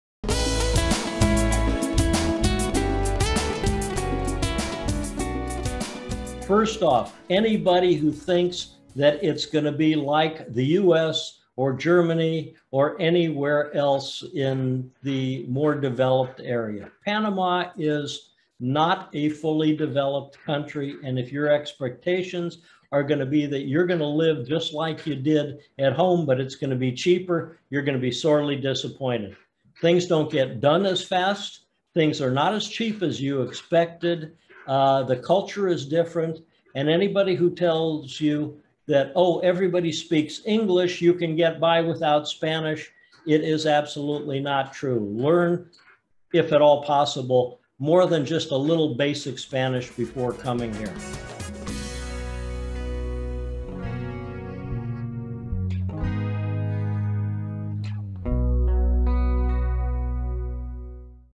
Steve Barnett Who Shouldn't Come to Panama
Long time Panama resident, six-time expat, world traveler, and avid motorcycle rider, Steve Barnett, shares his thoughts on who shouldn't come to Panama.